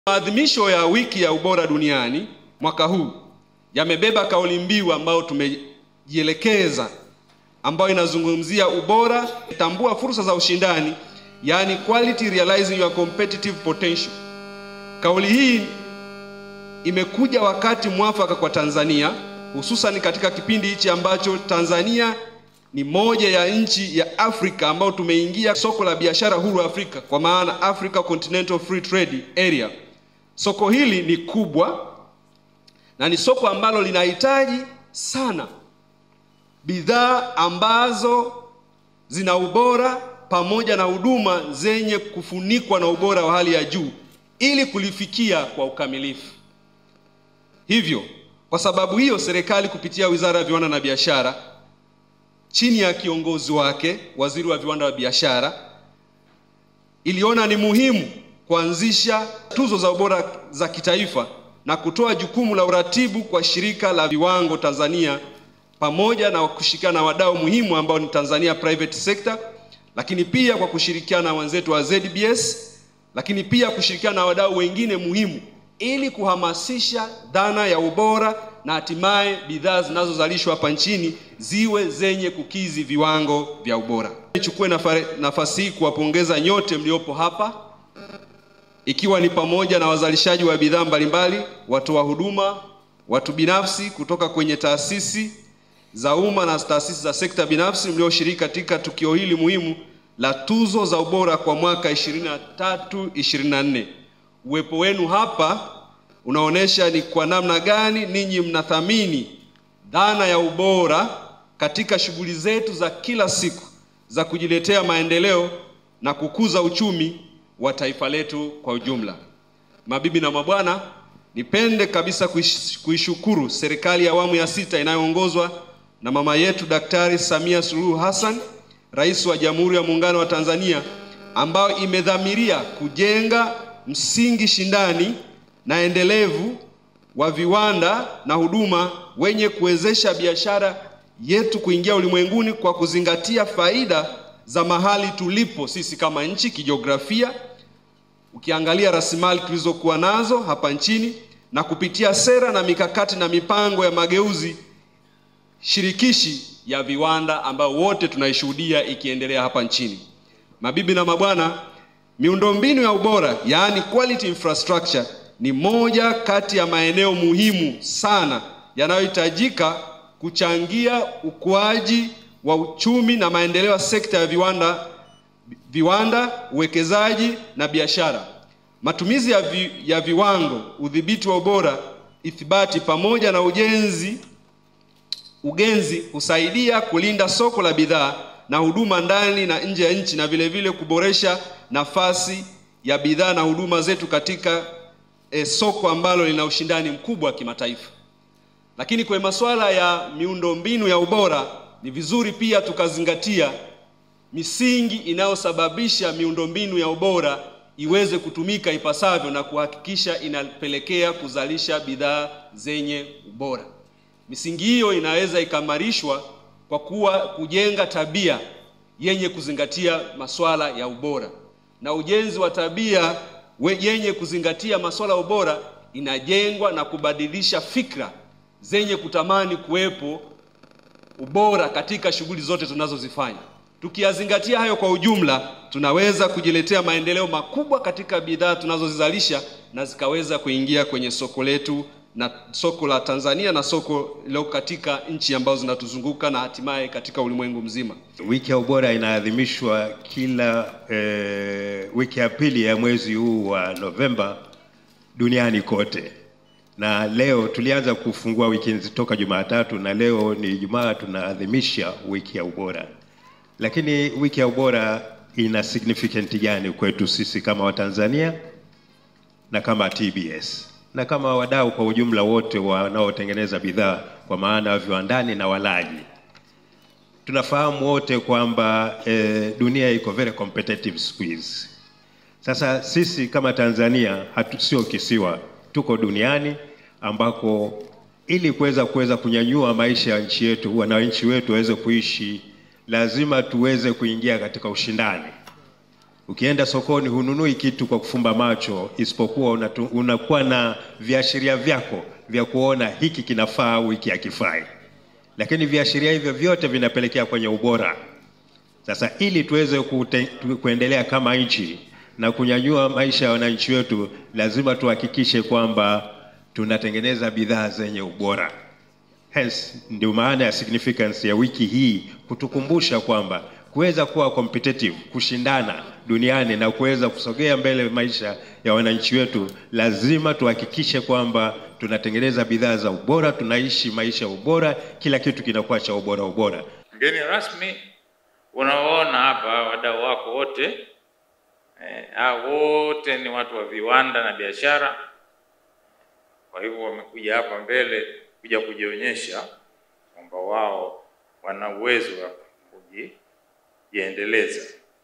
TBS YAADHIMISHA WIKI YA UBORA DUNIANI NA KUTOA TUZO KWA WAFANYABIASHARA NA WAJASIRIAMALI
MKURUGENZI wa Maendeleo ya Viwanda  kutoka Wizara ya Viwanda na Biashara  Bw.Sempeho Nyari amewataka wazalishaji wa bidhaa nchini na watoa huduma kutoka sekta za umma na binafsi kuunda miundombinu ambayo itasaidia kuzalisha bidhaa na huduma zenye ubora ili kuweza kushindana katika Soko la kimataifa
Ameyasema hayo leo Nov 10,2023 Jijini Dar es Salaam katika Maadhimisho ya kilele Cha wiki ya Ubora ambayo yameambatana na  utoaji Tuzo za Ubora ambazo zinatolewa na Shirika la Viwango Tanzania (TBS) yaliyofanyika katika Ukumbi wa Maktaba Chuo Kikuu cha Dar es Salaam
Amesema miundombinu ya ubora ni moja ya mambo muhimu yanayo changia katika kukuza uchumi na maendeleo ya Viwanda na Biashara  nchini kwa kuzalisha bidhaa na huduma zenye ubora.
“Maadhimisho ya wiki ya Ubora duniani mwaka huu yamebeba kauli mbiu ambayo tumeielekeza ambayo inazungumzia  UBORA TAMBUA UWEZO WAKO WA KUSHINDANA, kauli mbiu hii imekuja wakati muafaka kwa Tanzania hususani katika kipindi hiki ambapo ni moja ya nchi ambayo imeingia katika soko huru la biashara Afrika, soko hili ni  kubwa sana ambalo linahitaji sana bidhaa ambazo zina Ubora na huduma zilizofunikwa na ubora wa juu ili kulifikia kwa ukamilifu"Amesema
Aidha Bw.Sempeho amesema kuwa Serikali kupitia Wizara ya Viwanda na Biashara ilitambua ni muhimu kuanzisha Tuzo za Ubora za Kitaifa ambazo zitaratibiwa na Shirika la Viwango Tanzania (TBS) kwa kushirikiana na ZBS pamoja  Sekta binafsi na wadau wengine ili kuhakikisha bidhaa na huduma zinazotolewa zinakidhi ubora Kitaifa,kikanda na Umoja wa Afrika mashariki ili tuweze kulifikia soko.
"Nchi yetu ni moja ya nchi ambayo ipo kwenye Umoja wa kikanda yaani SADC na Umoja wa Afrika Mashariki EAC na Sasa hivi tumeingia soko huru la Afrika ni matumaini yangu tukizingatia zana ya ubora tutaingia sokoni kama washindani". Bw.Sempeho ameeleza.  Kwa Upande wake Mwenyekiti wa Bodi ya Wakurugenzi(TBS) Prof. Othman Chande ametoa wito kwa wazalishaji kuendelea kuzalisha bidhaa bora kila mwaka ili kuwa na utofauti kwa uboreshaji Viwango ili kukidhi mahitaji ya Soko.
Naye Mkurugenzi wa Shirika la Viwango Tanzania (TBS) Dkt Athuman Ngenya amesema wiki ya Ubora inamanufaa makubwa kwetu kwani itasaidia kuhamasishana kutengeneza bidhaa zenye ubora katika kushindana kwenye Soko la kimataifa.
"Tanzania sio kisiwa ili kunyanyua maisha ya Wananchi wetu lazima tutengeneze, lazima tuingie katika Ushindani ukienda soko hafumbi tu macho anaangalia unakua unachagua kipi ni bora  na kipi sio bora, wiki hii imekuja ili kukumbushana  juu ya kuhakikisha bidhaa zetu zinatengenezwa kwa ubora". Amesema Dkt.Ngenya
Tuzo hizo za Ubora  zimetolewa kwa Mara ya kwanza mwaka 2020/ 2021 ambapo hakukuwa na mwamko mkubwa katika Ushindani,washindi ambao wamepatikana katika Tuzo za 2022/2023 watapata fursa ya kufahamika nchini na kushiriki katika mashindano ya Tuzo za Ubora Afrika mashariki na ukanda wa SADC.Mkurugenzi wa Maendeleo ya Viwanda  kutoka Wizara ya Viwanda na Biashara  Bw.Sempeho Nyari akizungumza katika Maadhimisho ya kilele Cha wiki ya Ubora ambayo yameambatana na utoaji Tuzo za Ubora ambazo zimetolewa na Shirika la Viwango Tanzania (TBS) yaliyofanyika leo Novemba 9, 2023 katika Ukumbi wa Maktaba Mpya Chuo Kikuu cha Dar es Salaam
Tufuatilie kwenye mitandao ya kijamii